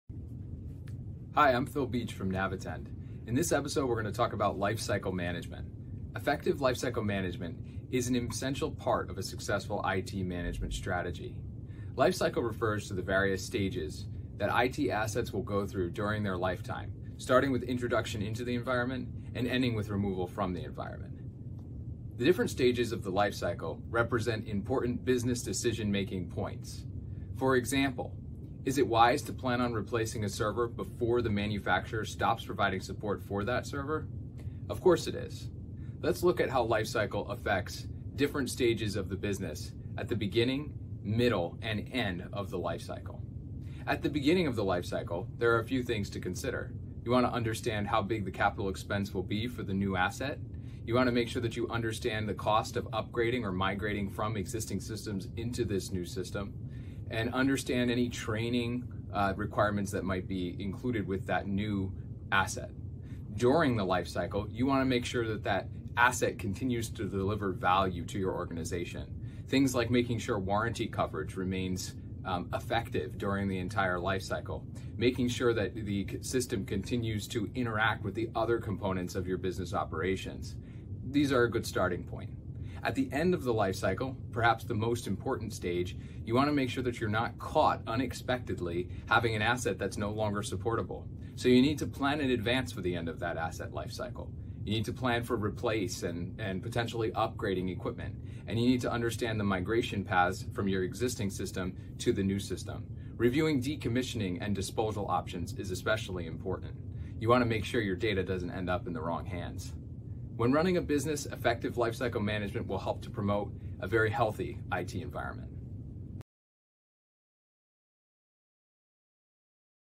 IT Lifecycle Management
Effective lifecycle management is an essential part of a successful IT management strategy.

“Lifecycle” refers to the various stages that IT assets will go through during their lifetime, starting with the introduction into your business environment and ending with removal from your environment.